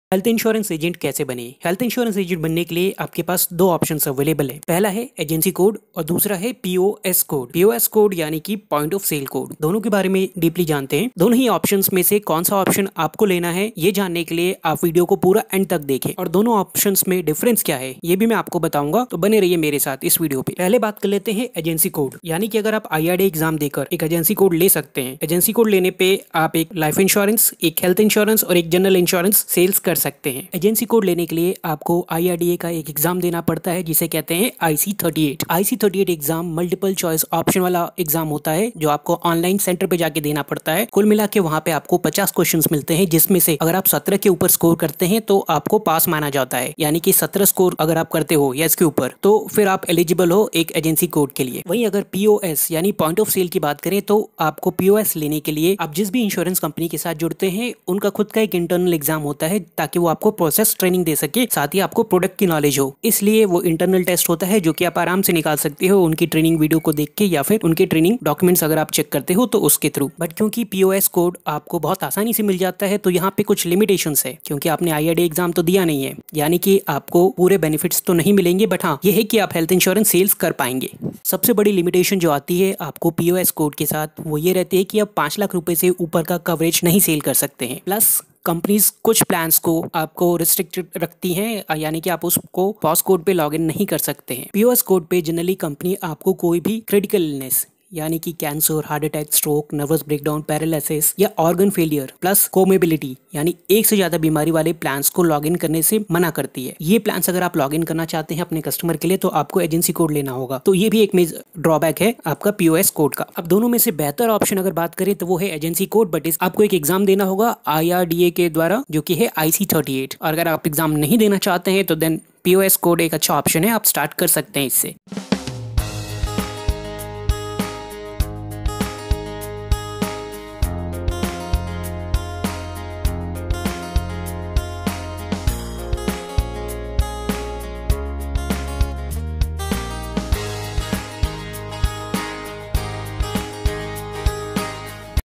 health insurance agent kaise bane| health insurance agent comission| Exam
If you want to become a health insurance agent Please ping me on whattsapp: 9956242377.
In this vedio I have explained how you can become a health insurance Agent. if you to want to make some money and you are into life or gerneral insurance then health insurance is a good option as you already have your own customer base and along with this you don,t have to give any exam. for freshers POS code qill be a good option to start with. check this video and you will come to know how to become a health insurance agent.